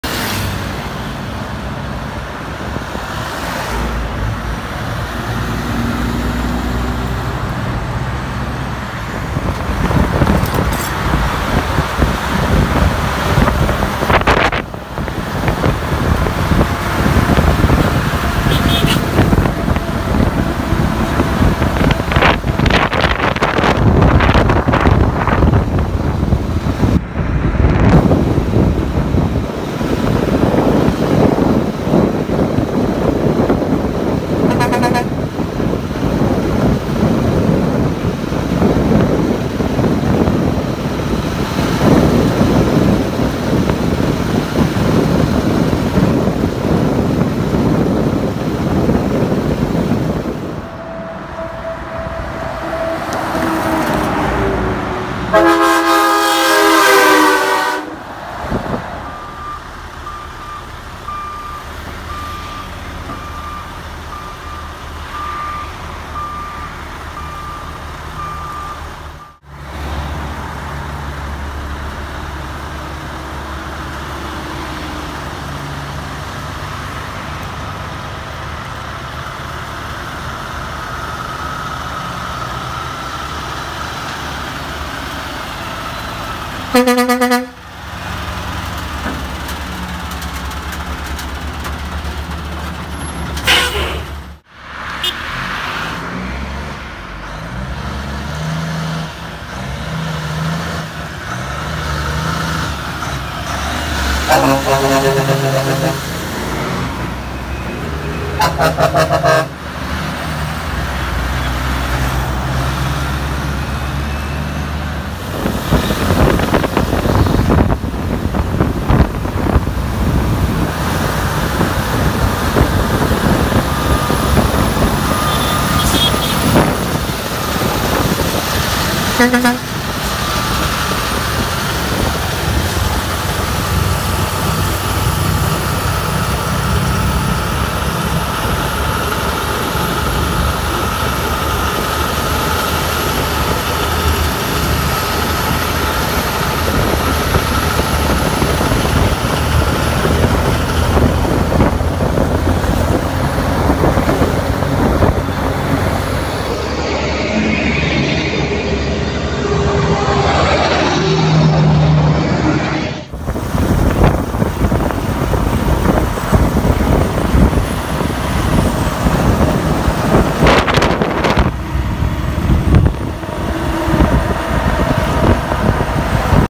Kenworth y más Kenworth!🇨🇴🚛
Todo tipo de tractomulas Kenworth (T800, T680) una completa locura!!😍🔥